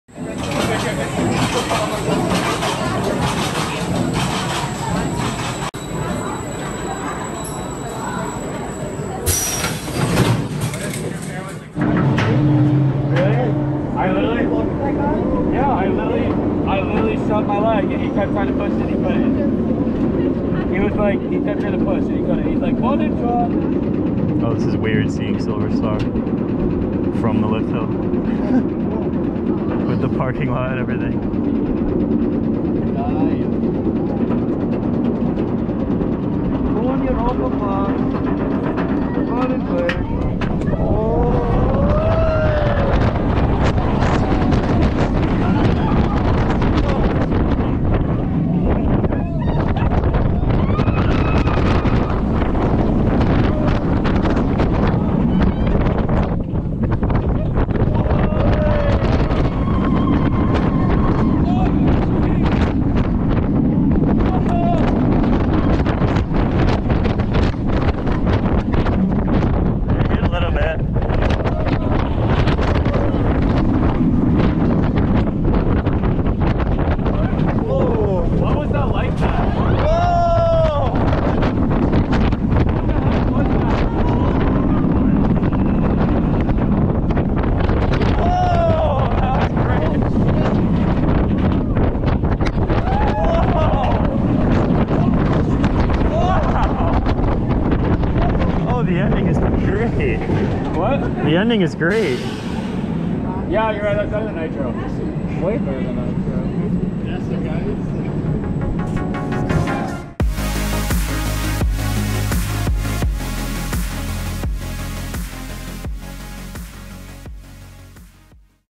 Silver Star, Europa Park Roller Coaster - Back Row POV
Silverstar is the tallest roller coaster at Europa Park. It's 240 foot lift hill drops you right over the entrance road, and creates quite an amazing centerpiece for the parking lot. It is one of the most well-known B&M Hyper coasters because of it's location, and it was also one of the first in the World, and the first in Europe. Bolliger and Mabillard rides are known for their smooth roller coasters, and Silver Star is no exception, it still runs pretty well for its 20+ years of age.

The attraction is one of the few rides inside Europa Park that is not MACK-manufactured.

Hyper coaster, Silver Star, Europa Park, Rust, Germany. 4K On-Ride, POV Back Row.